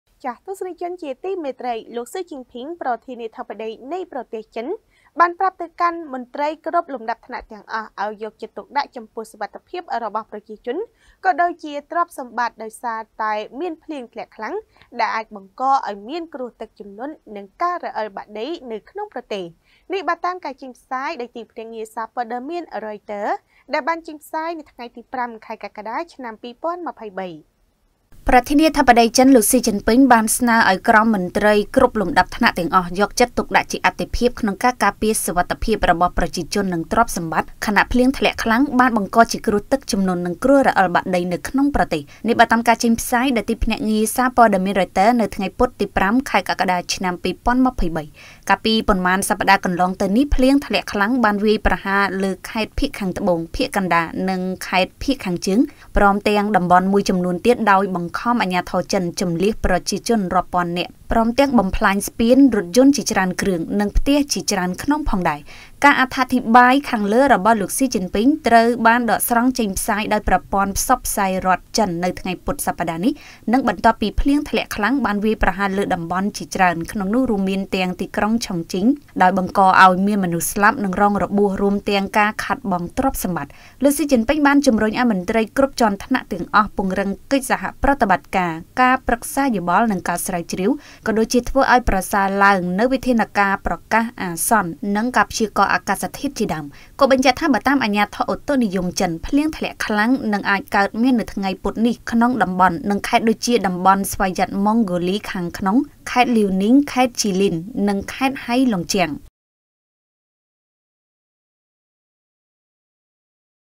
លោក ស៊ី ជិនពីង ប្រាប់ឱ្យក្រុមមន្ត្រីចិន ពង្រឹងវិធានការបង្ការគ្រោះទឹកជំនន់ ចំពោះមុខភ្លៀងធ្លាក់ខ្លាំង
* រាយការណ៍ដោយកញ្ញា ថាន  រស្មី និងកញ្ញា យឹម ចាន់ស្រីវីន